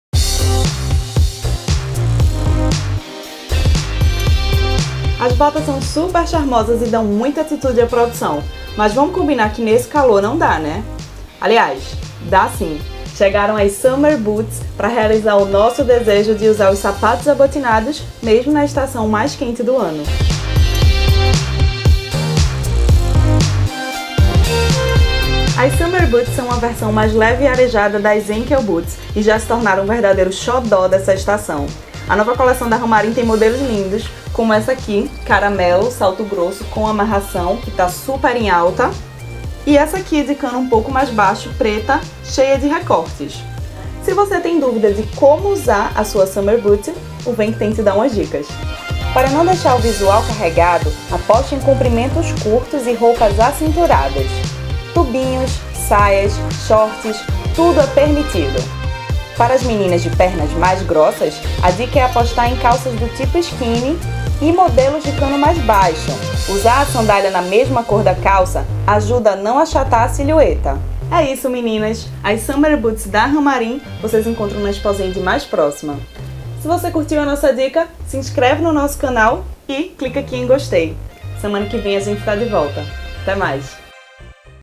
Vem Que Tem - Summer boots
Conheça a versão dos sapatos abotinados para o verão e saiba como usar as Summer Boots.